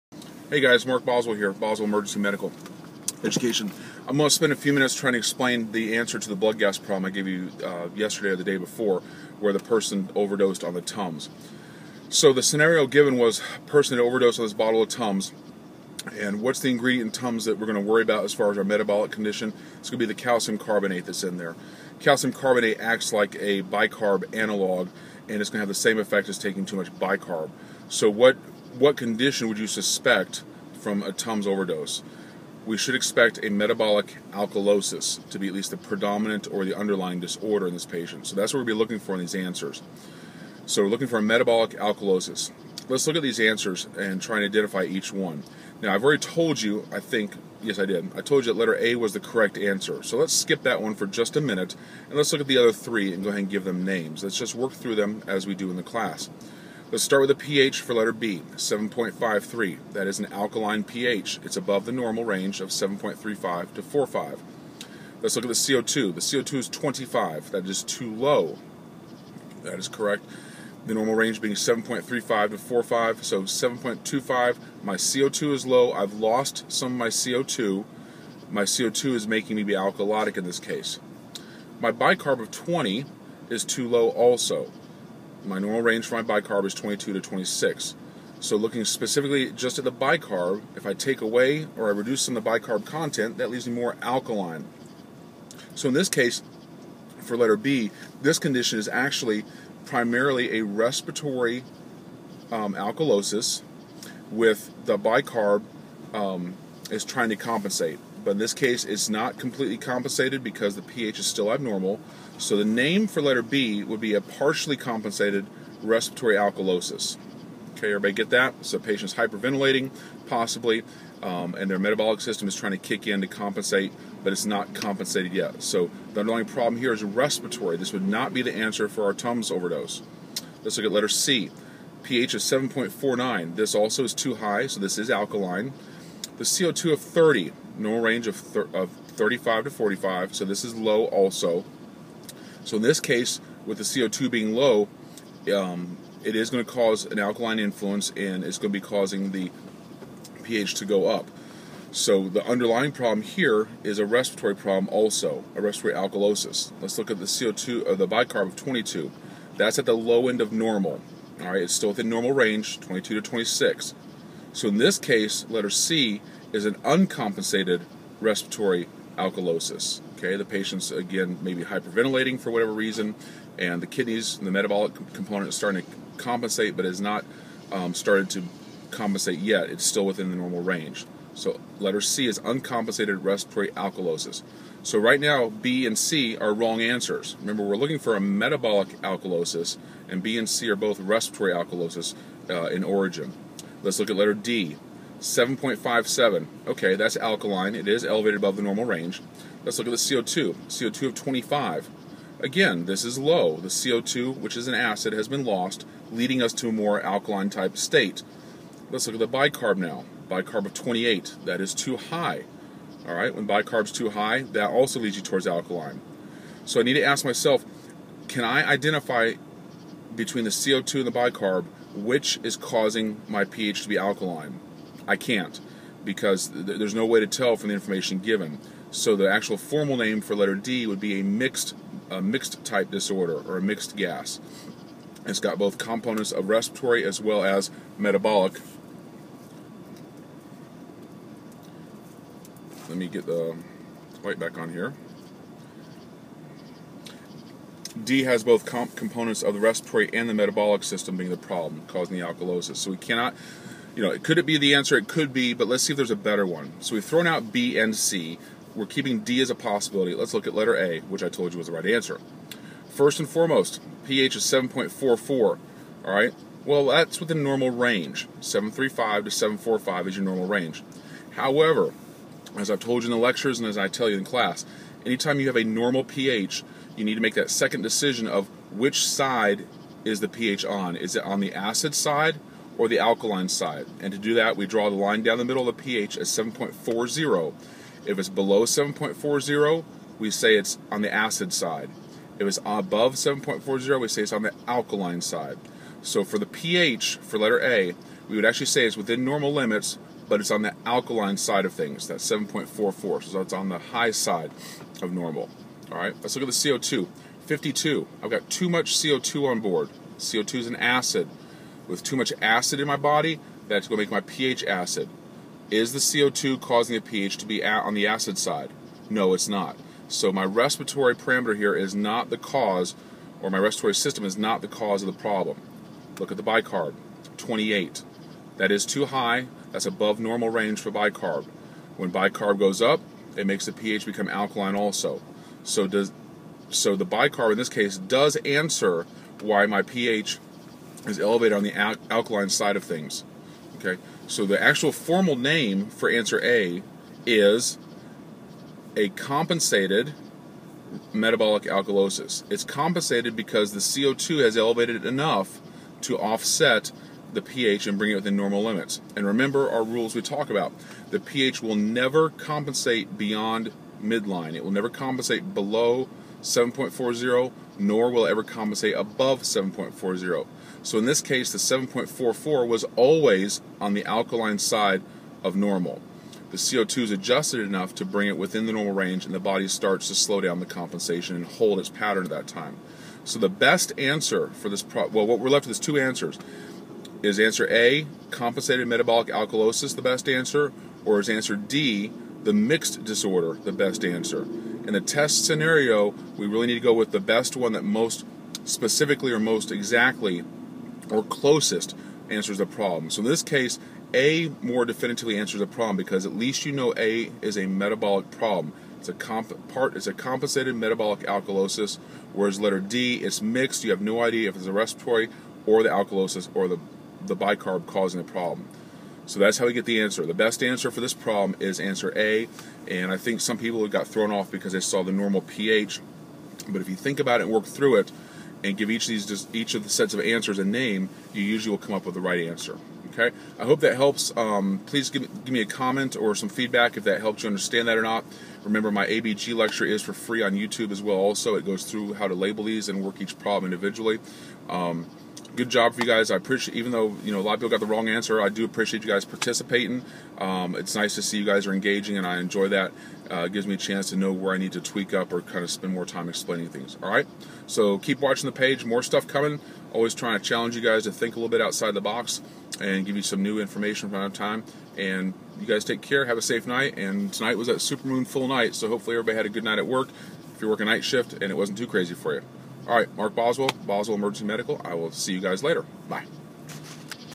Video Answer/Discussion on TUMS OD and ABG values
A Video discussion on the correct answer to the TUMS overdose problem and what ABG values would be expected.